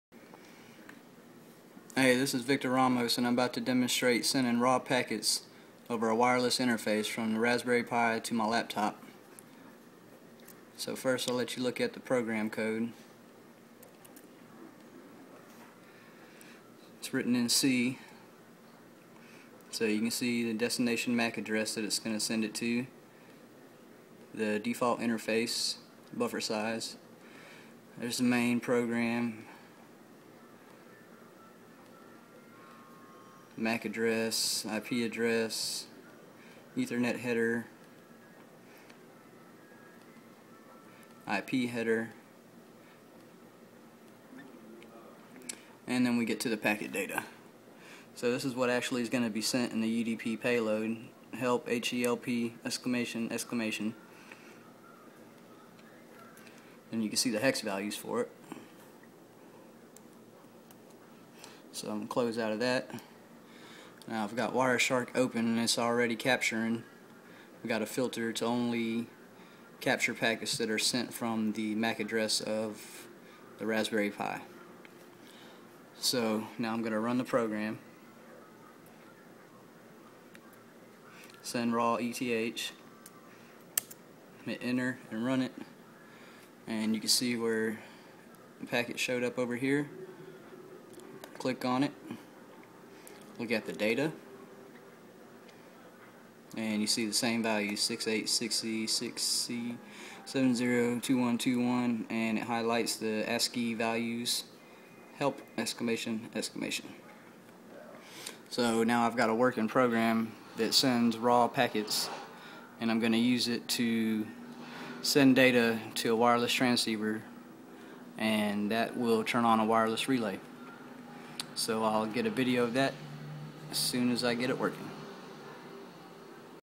Sending Raw Packets From Raspberry Pi
This video shows a program that sends raw UDP packets through a wireless interface from the Raspberry Pi to my laptop. The source code shows the values sent and Wireshark is used to verify that the packet is sent and also shows the data contained in the UDP payload. I will use a modified version of this program to activate solid state relays in the near future...